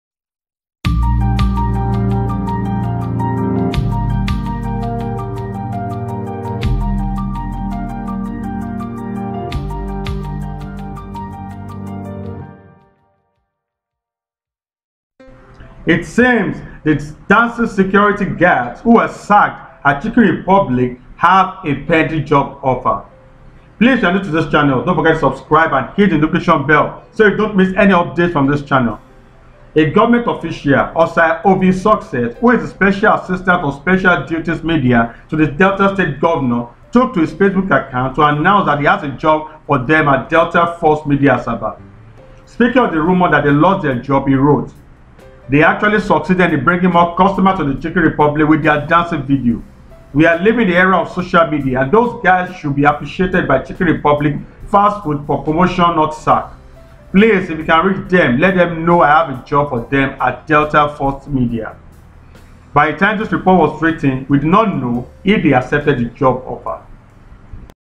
Dancing Chicken Republic security guards allegedly offered jobs at Delta First Media
It seems the dancing security guards who were sacked at chicken republic have a pending job offer.